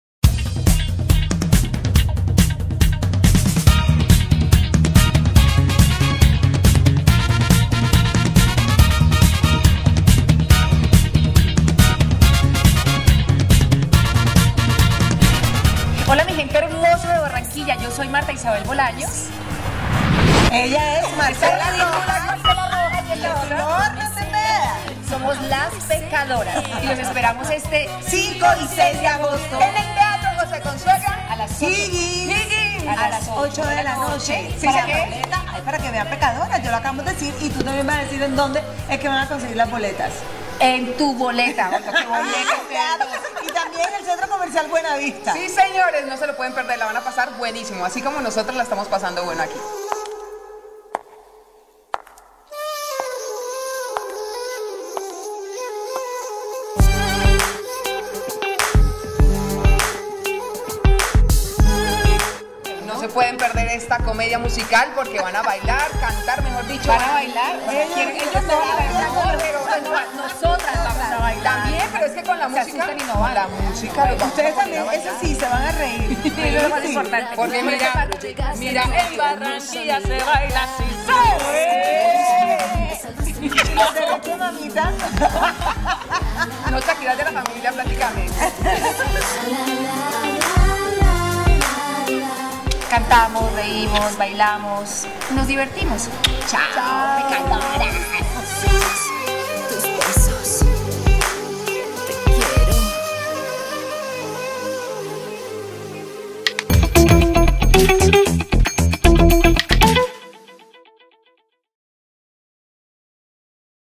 Pecadoras Funciones Barranquilla :: Spot ::
:: Pecadoras ::

Con:

Lorna Cepeda
Martha Isabel Bolaños
Marcela rojas

Fecha: 5 y 6 de Agosto

Ciudad : Barranquilla

Lugar : Teatro José Consuegra Higgins

Producción :  Artes Y Espectáculos Producciones